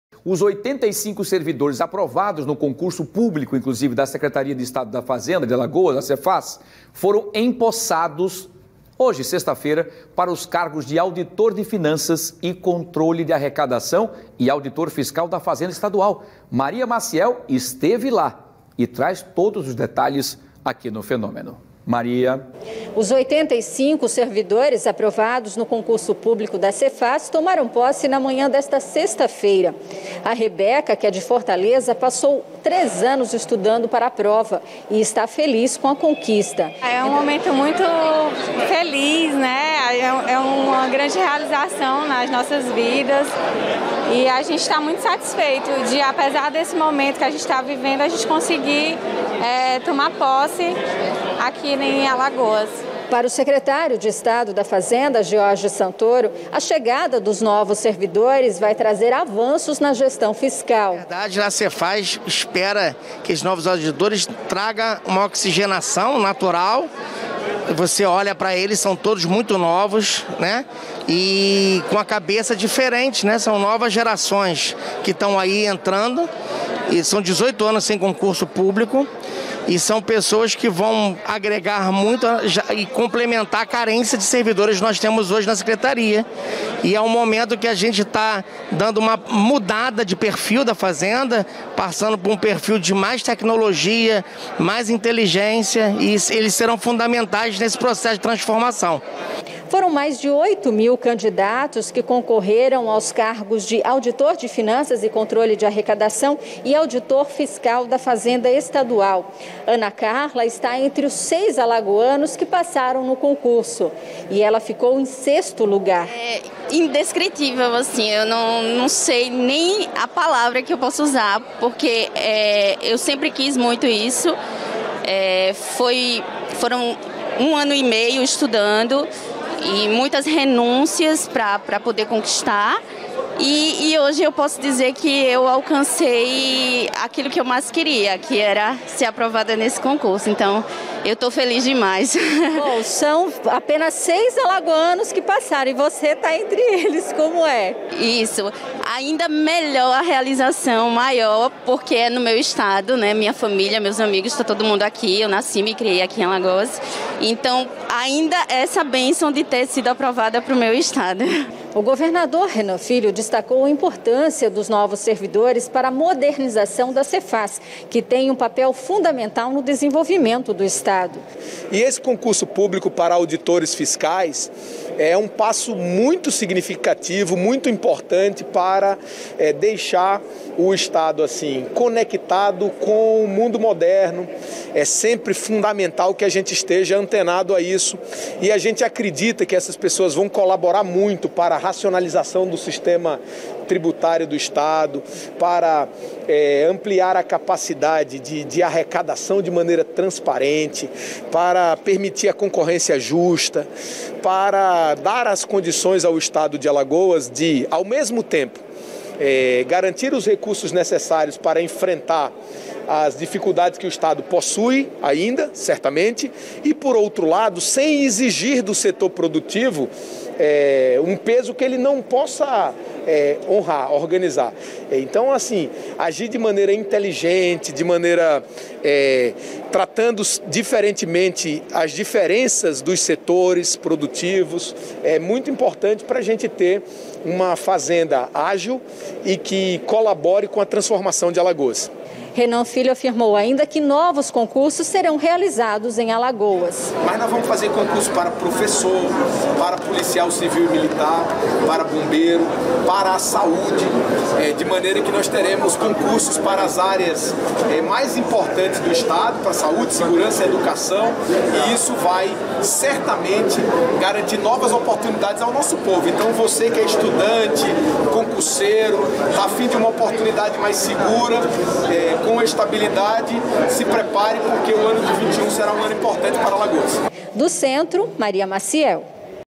Governo de Alagoas empossa novos auditores da Secretaria da Fazenda
A solenidade aconteceu na manhã desta sexta-feira.